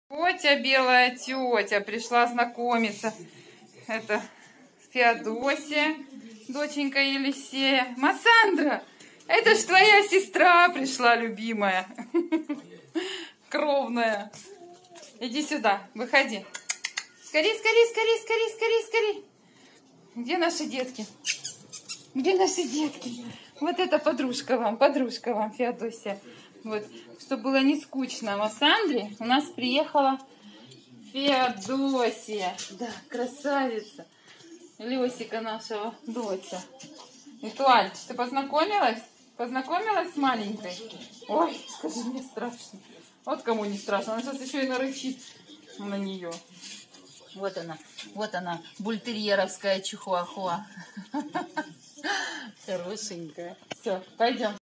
первое знакомство Феодосии с Этуаль и Массандрой СИМФЕРОПОЛЬ Кастропуло Крым продам щенков
Будни наших чихуахуа
Влог красивых чихуахуа, Симферополь, Крым,
Питомник чихуахуа Кастропуло,Kastropulo